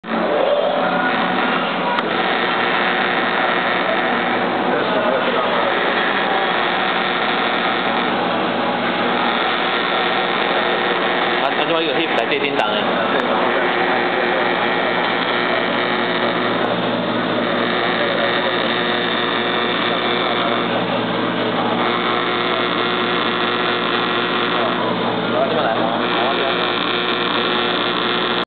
KA-B6, Automatic PCB Brushing and Lead cutting Machine
KAB6
Automatic PCB Lead cutting and Brushing Machine

KAB6 inner rotation

FEATURES

PCB Working Range: 10mm ~ 300mm.
Brush is optional selected for cleaning leads adhered after cutting.
Available for connecting with Wave soldering machine in mass production.
PCB Wrap proof device is equipped.
The height of Back/Front conveyors are adjustable to meet various kind of connection.
The Tungsten Carbide Blade is excluded in the machine.
Net Weight:200Kgs

    SPECIFICATIONS

Dimension:1160(L) x 730(W) x 800(H),Power:220V 50/60HZ
RPM of Spindle:4500
HP:3/4HP
Feeding Speed:0 - 3m (Adjustable)
Min. Lead Height:1.0mm Min
MAX.PCB Working Size:500mm(L) x 300mm(W)
RPM of Brushing:600 RPM/1/2HP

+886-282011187